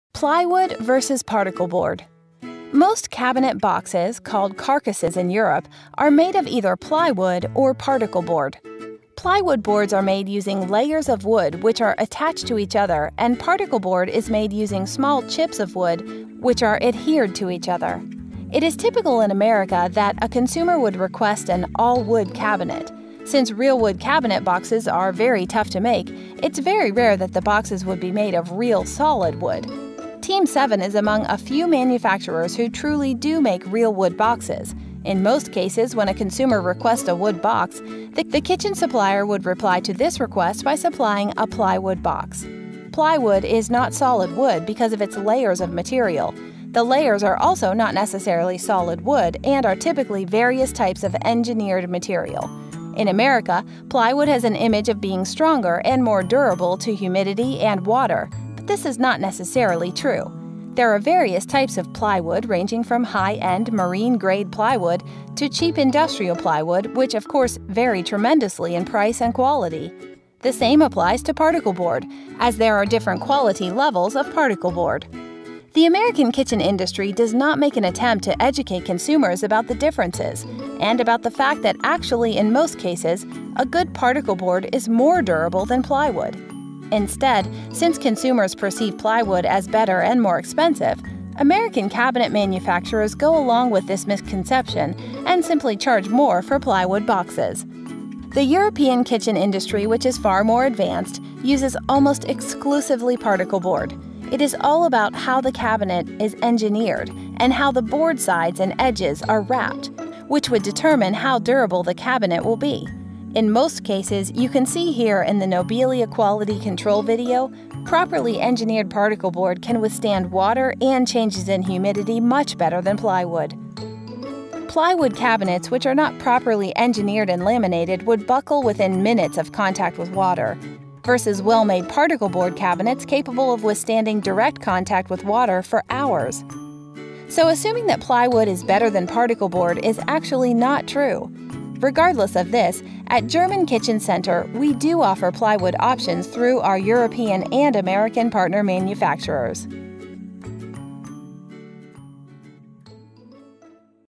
Plywood Vs. Particle Board
Most cabinet boxes (Called carcasses in Europe) are made of either plywood or particle board.

Plywood boards are made using layers of wood which are attached to each other and particle board is made using small chips of wood which are adhered to each other.

It is typical in America that a consumer would request an “all wood” cabinet. Since real wood cabinet boxes are very tough to make it is very rare that the boxes would be made of real solid wood. Team7 is among a few manufacturers who truly do make real wood boxes. In most cases when a consumer requests a wood box, the kitchen supplier would reply to this request by supplying a plywood box.

Plywood is not solid wood, because it is layers of material. The layers are also not necessarily solid wood and are typically various types of engineered material. In America, plywood has an image of being stronger and more durable to humidity and water, but this is not necessarily true.

There are various types of plywood ranging from high end marine grade plywood to cheap industrial plywood, which of course vary tremendously in price and quality. The same applies to particle board as there are different quality levels of particle board.

The American kitchen industry does not make an attempt to educate consumers about the differences and about the fact that actually in most cases a good particle board is more durable than plywood. Instead, since consumers perceive plywood as better and more expensive, American cabinet manufacturers go along with this misconception and simply charge more for plywood boxes!

The European kitchen industry which is far more advanced, uses almost exclusively particle board. It is all about how the cabinet is engineered and how the board sides and edges are wrapped which would determine how durable the cabinet will be. In most cases, as you can see here in the Nobilia Quality control video, properly engineered particle board can withstand water and changes in humidity much better than plywood.

Plywood cabinets which are not properly engineered and laminated would buckle within minutes of contact with water, verses well made particle board cabinets capable of withstanding direct contact with water for hours.

So assuming that plywood is better than particle board is actually not true!

Regardless of this, at German Kitchen Center we do offer plywood options through our European and American partner manufacturers.